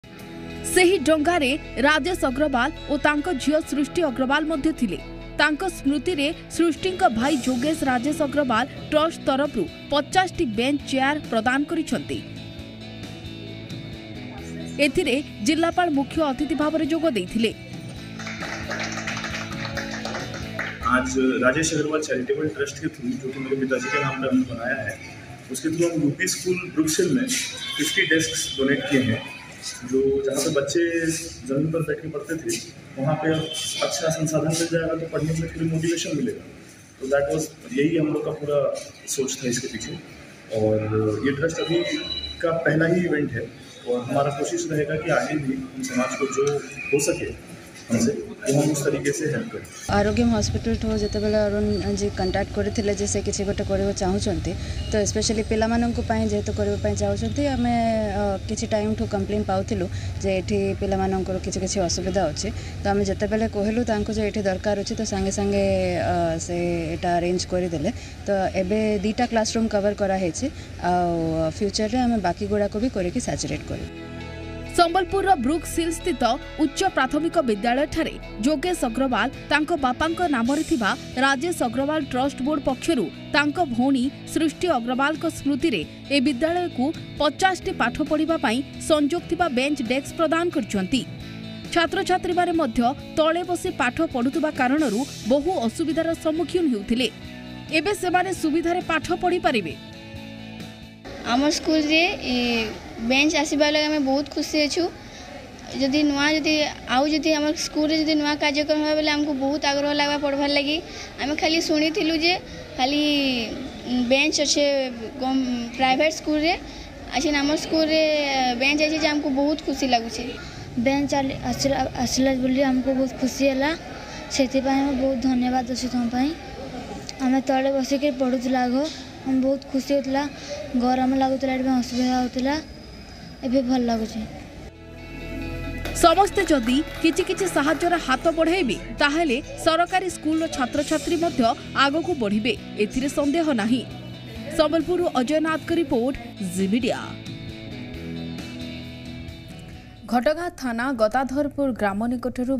Youth Donates 50 Desks To Govt School In Sambalpur In Memory Of His Sister | ZeeOdishaNews
ସରକାରୀ ବିଦ୍ୟାଳୟକୁ 50ଟି ବେଞ୍ଚ ଡେସ୍କ ପ୍ରଦାନ କଲେ ଯୁବକ |
About Channel:-